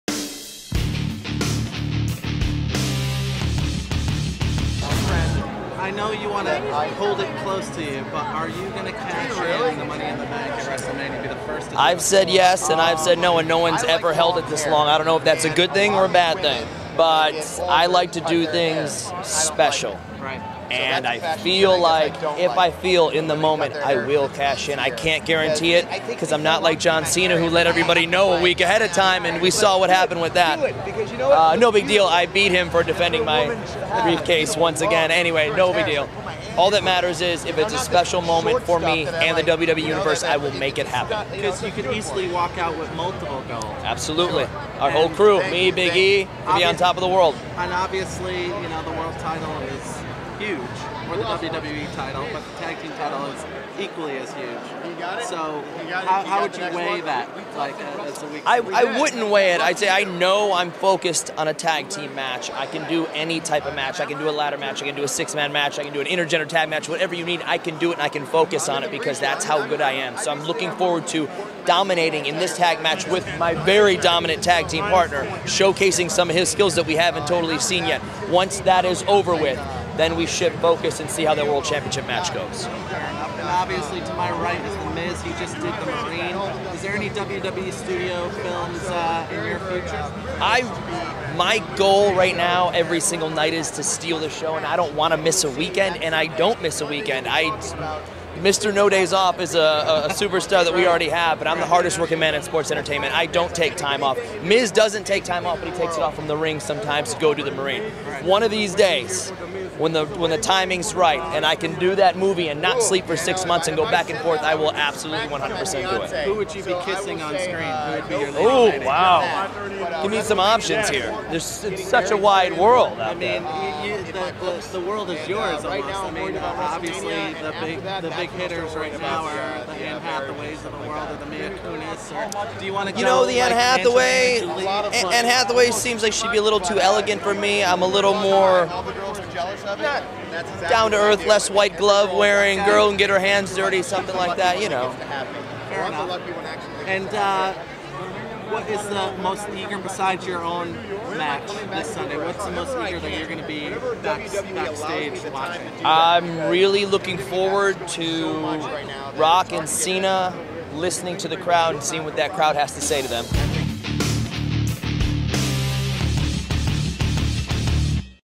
WWE Heavyweight Champion Dolph Ziggler Talks WrestleMania 29 And Superstar Challenge - Gamerhubtv
In New York City before WrestleMania 29, WWE Superstar Dolph Ziggler walked the red carpet to raise money for Hurricane Sandy Relief. Ziggler talks WrestleMania, Superstar Challenge and video games in this exclusive interview.